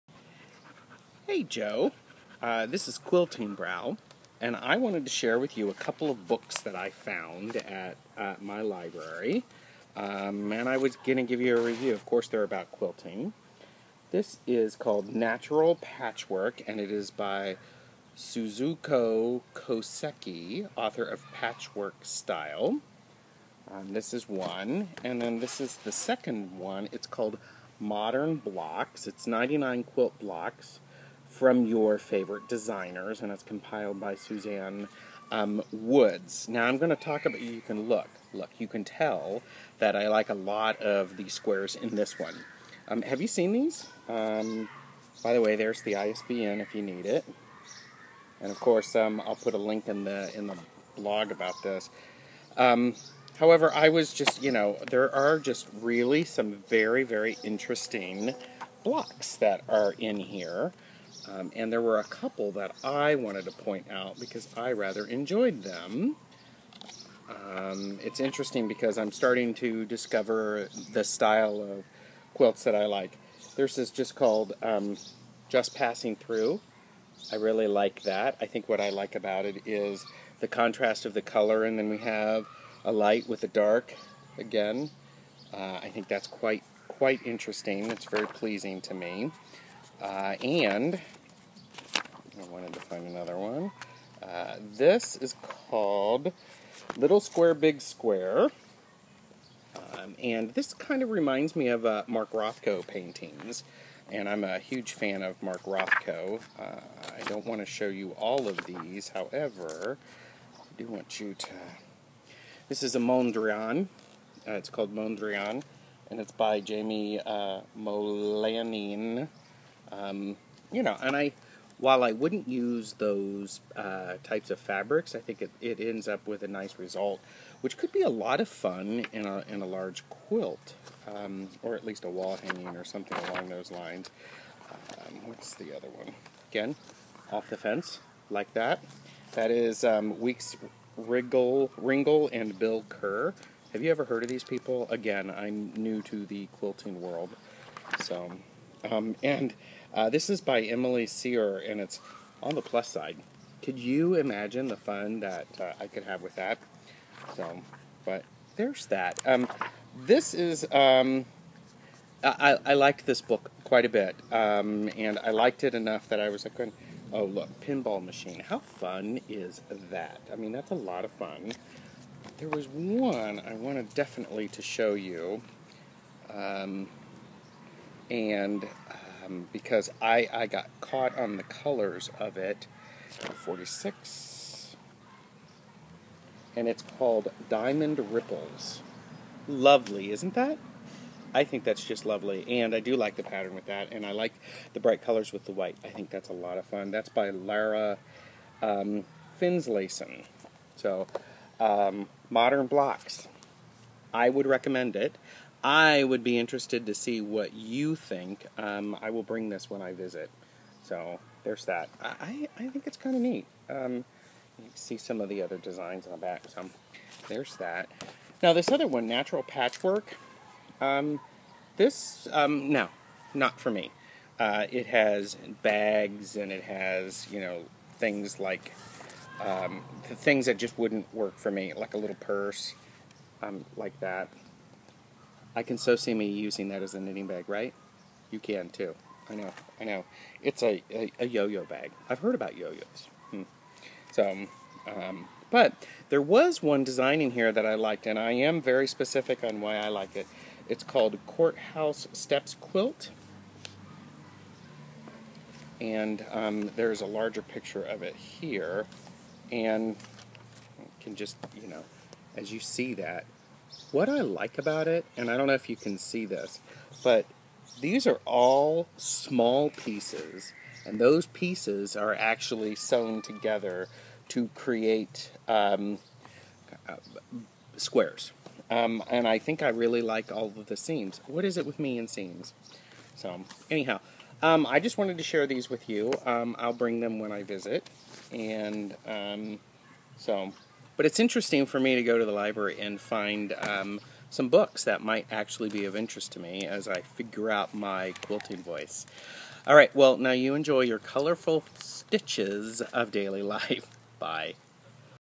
Quiltingbrow reviews two books
I review Modern Blocks compiled by Susanne Woods and Natural Patchwork  by Suzuko Koseki